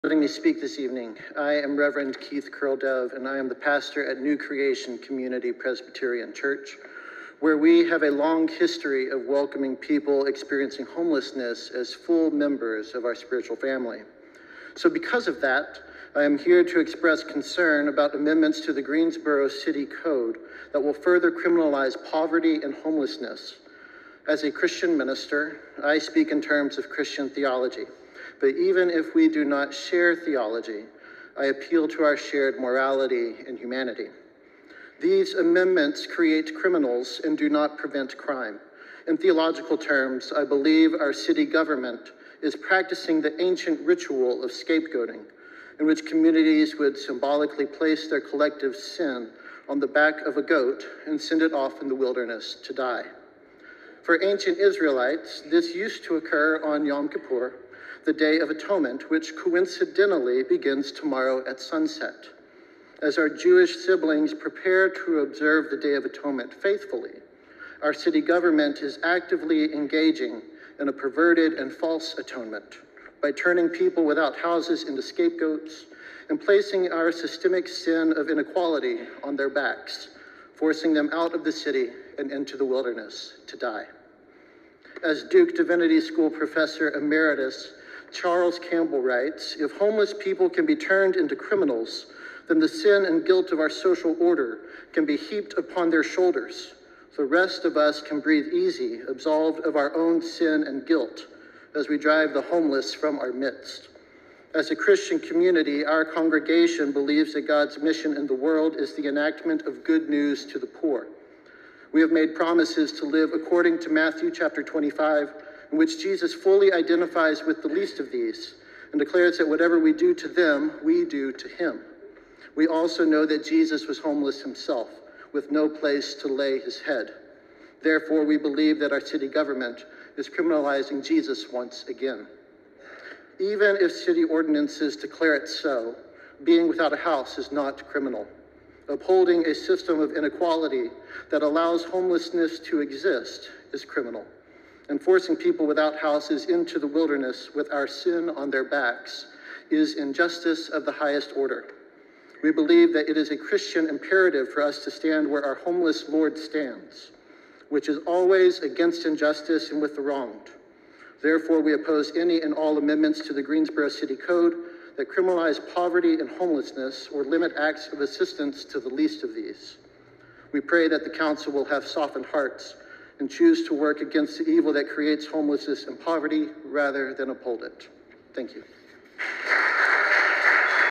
Criminalizing poverty is scapegoating the poor
Rev. Keith Curl-Dove speaks against the anti-houseless policies that scapegoat the poor for the crimes of the wealthy.